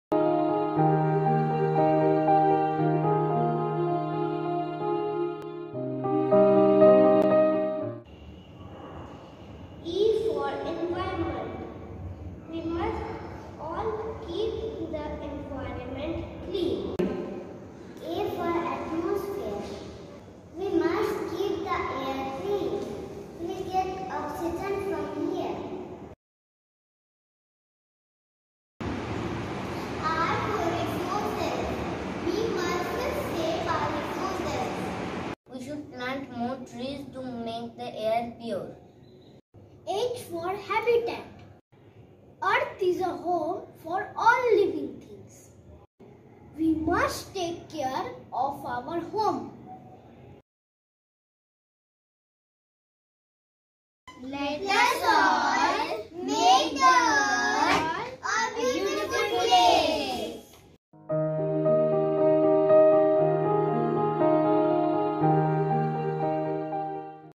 Earth Day 2022 | DPS Durgapur | Earth Day Celebration
DPS Durgapur believes a green future is a prosperous future and celebrates the day, promoting this year's theme, 'Invest in our Planet'.